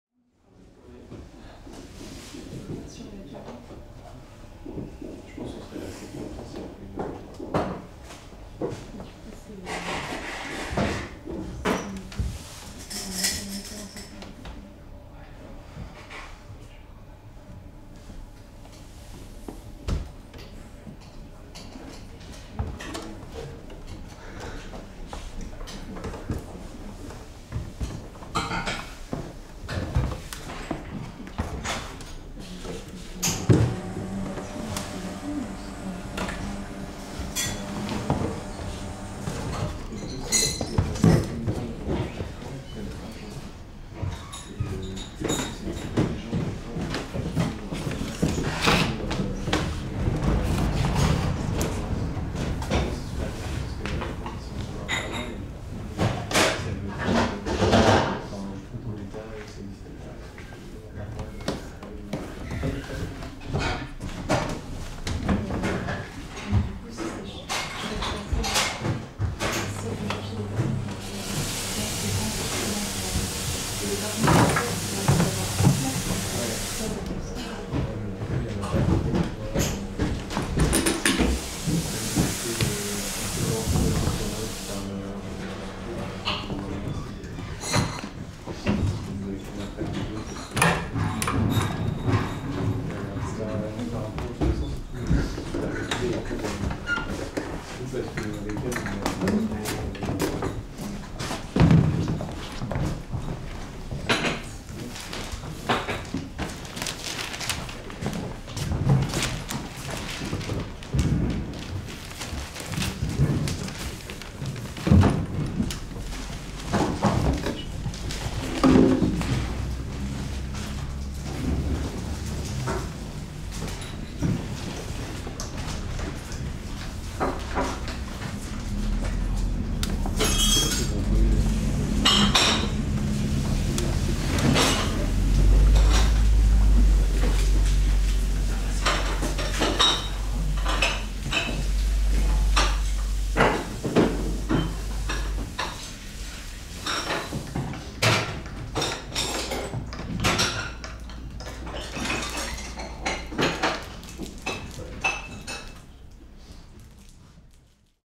CHINATSU KOSAKATANI #2
Almost like a character out of a film, Chinatsu inhabits the space of L’Illustration, an artists’ cafe with a serene atmosphere. Light and mischievous she sneaks from one table to the next and creates a game with the regulars of the day. Richard, the owner gazes fondly at the scene, turns the percolator back on and the afternoon continues.

Presque en personnage de cinéma, Chinatsu habite l’espace de l’Illustration, ce café d’artistes à l’atmosphère sereine. Sa présence glisse d’une table à l’autre, légère et espiègle et c’est un jeu avec les habitués du jour qui s’écrit. Richard, le patron couve la scène de ses yeux doux, rallume le percolateur et l’après-midi suit son cours.

Director/Image

Thibaut Ras - Bertrand Guerry - Marie Pons

Production

AllWeCanDo